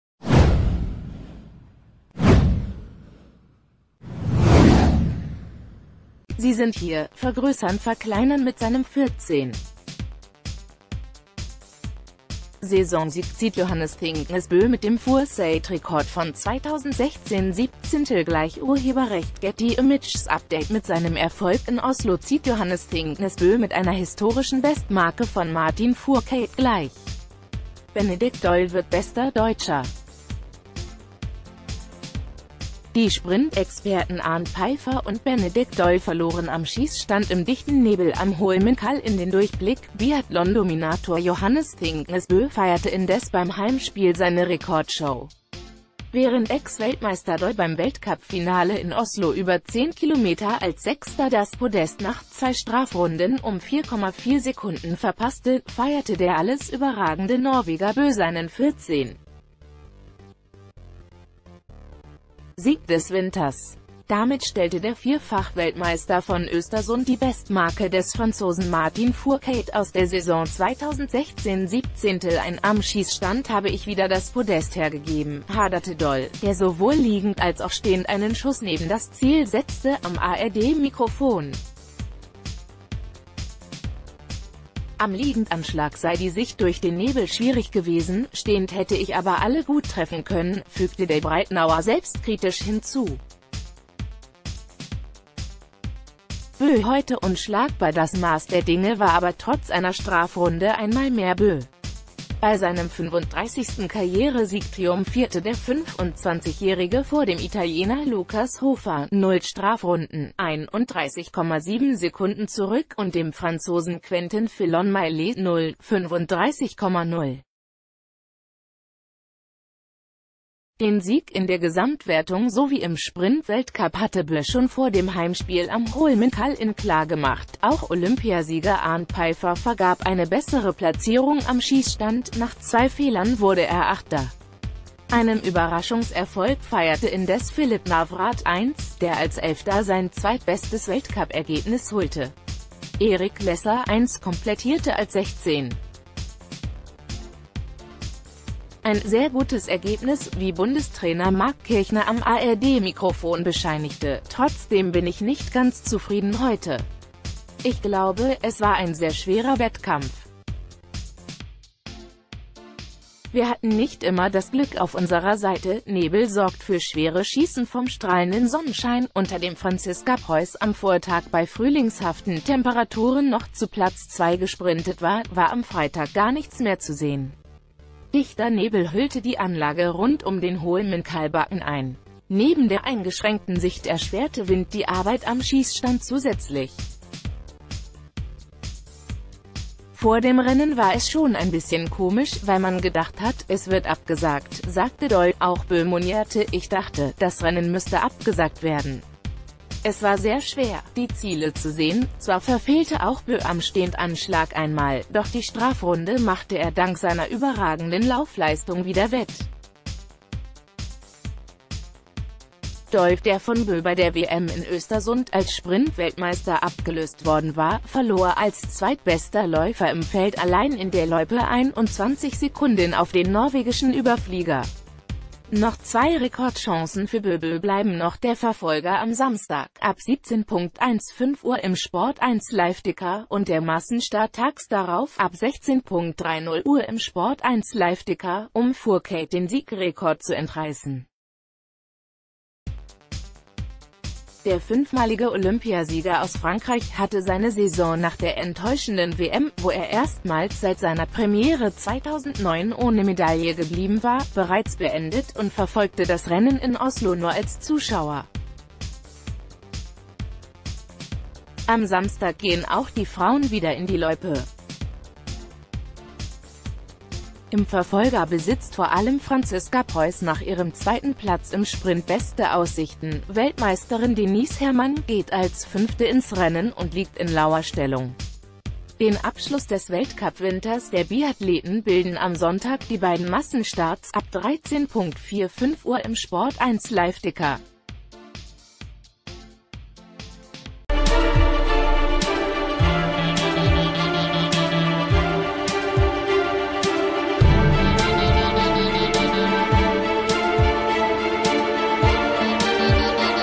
Biathlon: Thingnes Bö stellt Fourcade-Rekord ein - Benedikt Doll bester Deutscher
Sie sind hier: vergrößernverkleinern Mit seinem 14. Saisonsieg zieht Johannes Thingnes Boe mit dem Fourcade-Rekord von 2016/17 gleich © Getty ImagesUpdate Mit seinem Erfolg in Oslo zieht Johannes Thingnes Bö mit einer historischen Bestmarke von Martin Fourcade gleich. Benedikt Doll wird bester Deutscher. Die Sprint-Experten Arnd Peiffer und Benedikt Doll verloren am Schießstand im dichten Nebel am Holmenkollen den Durchblick, Biathlon-Dominator Johannes Thingnes Bö feierte indes beim Heimspiel s...